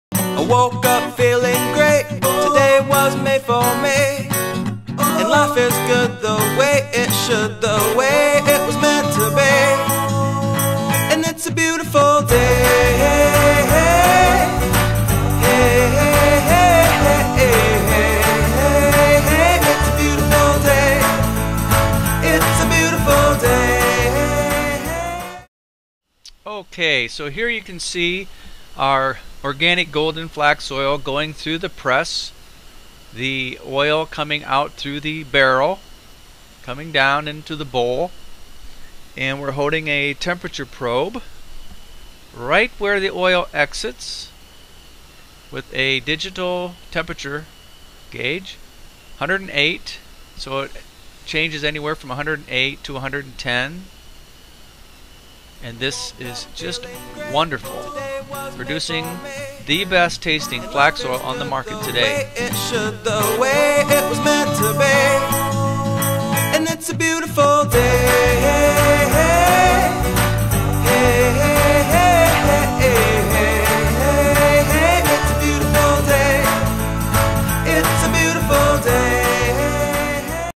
Foods Alive Artisan Cold Pressed Organic Oils (Chia, Flax, Hemp & Black Sesame)
Foods Alive has the best tasting Artisan Cold-Pressed Oils on the market! The reason why is because they are pressed in small batches while only being heated around 108 degrees. You won't find that anywhere else on the market. Be sure to take a gander at our lineup of Super Dressings, Super Foods, and Glassware that will knock your socks off!